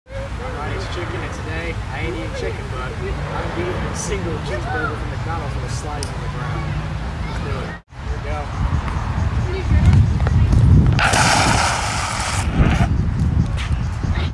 Sliding while eating a McDonald’s cheeseburger
I was sliding with my gear on and decided to eat a McDonald’s cheeseburger while doing it.